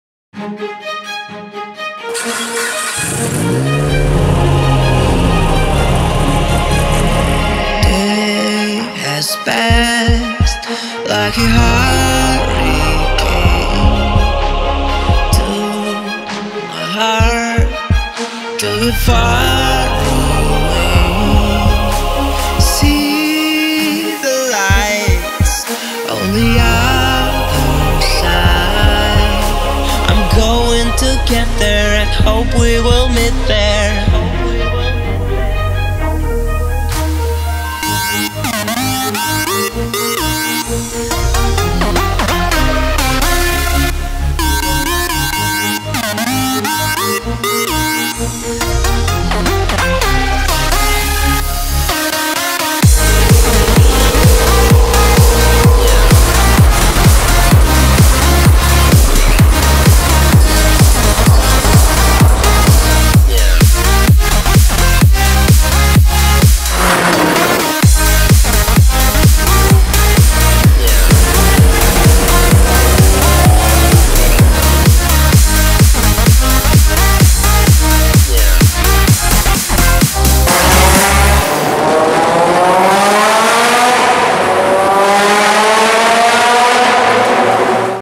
JDM Night Out
Maxson Goh Films a group of JDMs going on a night drive around Singapore consisting of a Honda S2000, Nissan Skyline R34, Mitsubishi Lancer Evolution IX & X and aHonda Civic Type R.